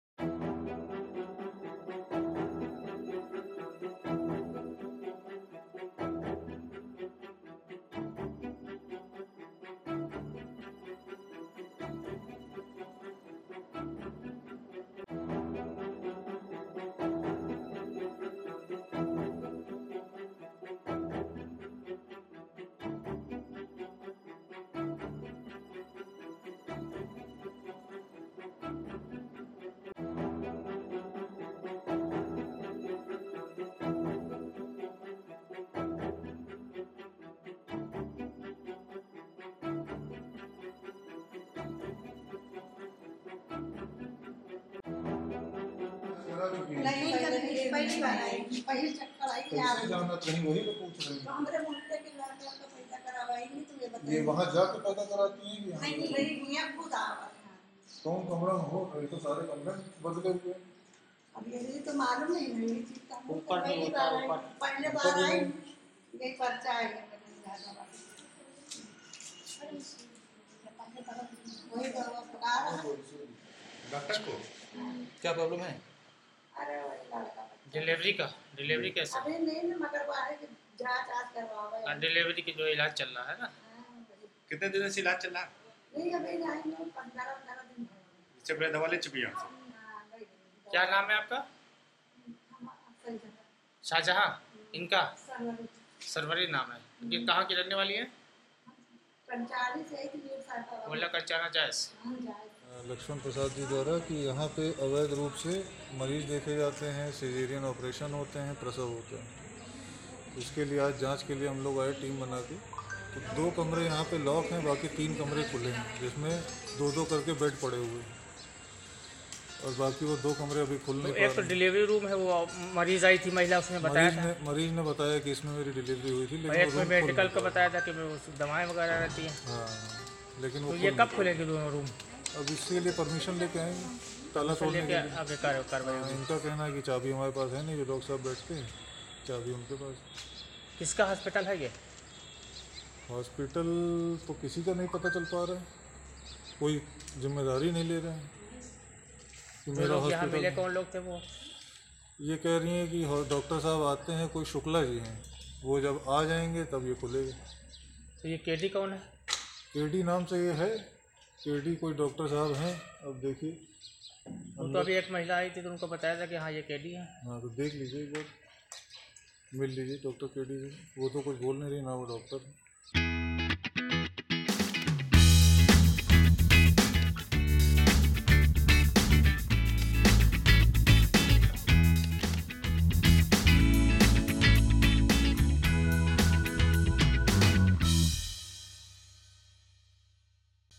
अस्पताल में अवैध रूप से चल रहा था डिलीवरी रूम व मेडिकल स्टोर, स्वास्थ्य विभाग की टीम ने मारा छापा
सुलतानपुर-रायबरेली नेशनल हाईवे पर एचडीएफसी बैंक के सामने मुहल्ला गांजा में एक निजी मकान में लंबे समय से चल रहे अस्पताल में सोमवार को सीएचसी प्रभारी डा. एचपी यादव व उनकी टीम ने छापा मारा। दोपहर बाद शुरू हुई कार्रवाई शाम पांच बजे तक चली। अस्पताल में एक दर्जन बेड, मरीजों की जांच व ओपीडी पाए गए। अस्पताल में आपरेशन रूम व मेडिकल स्टोर में ताला बंद था।